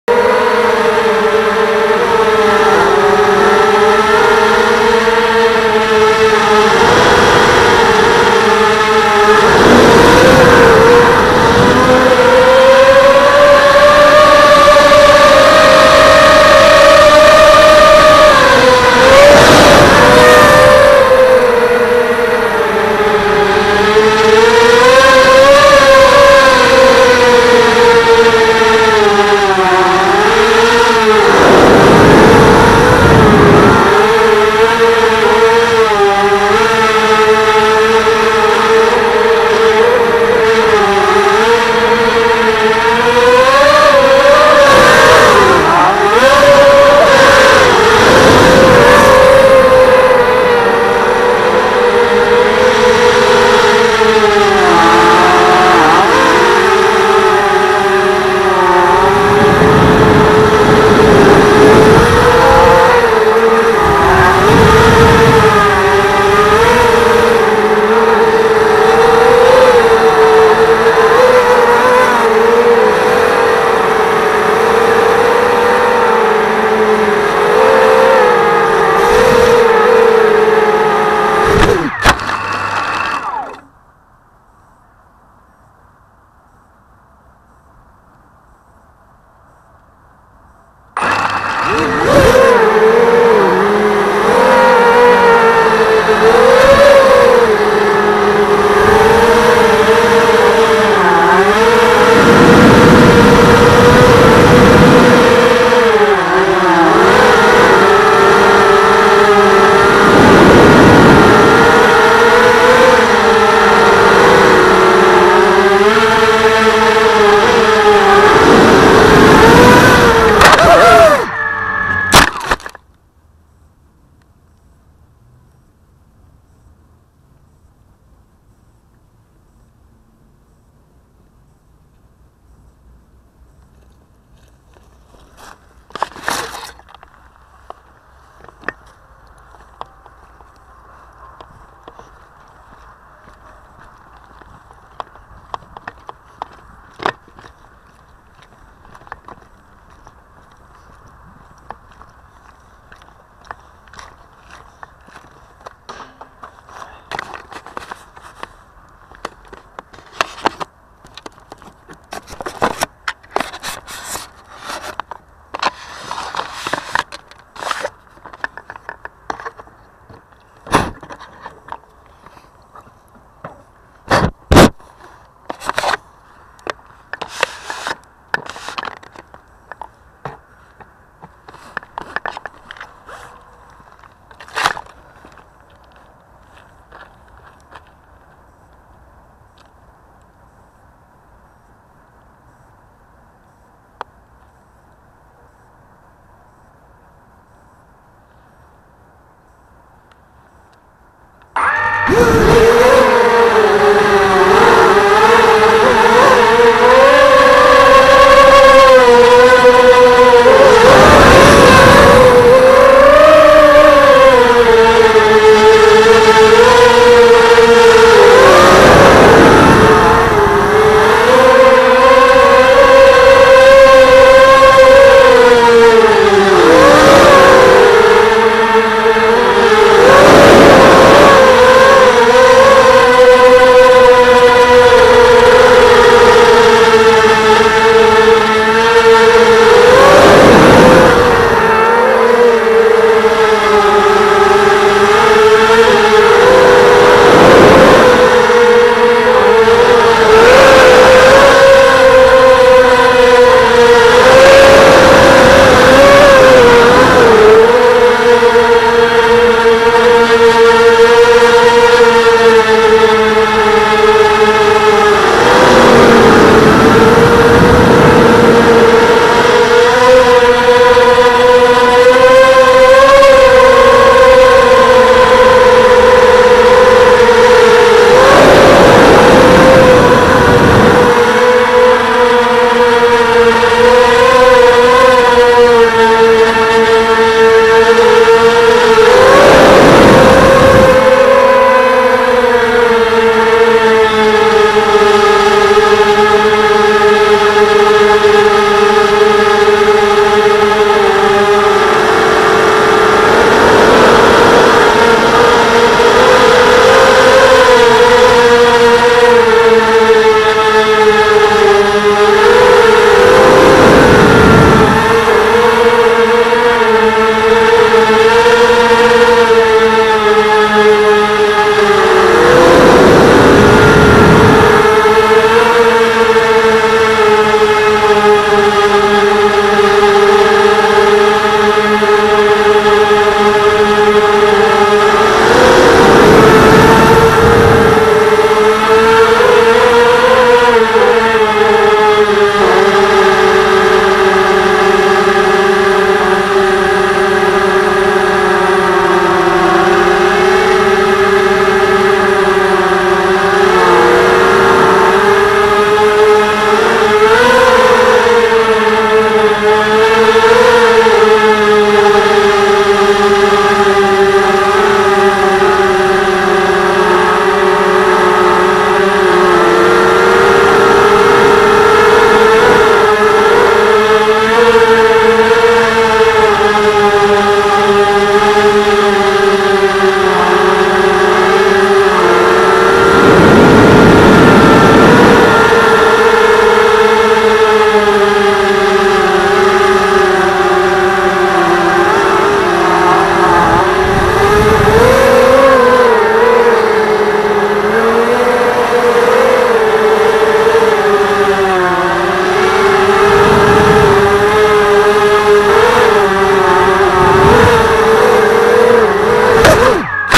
TBS Gemini
My first flights with the TBS Gemini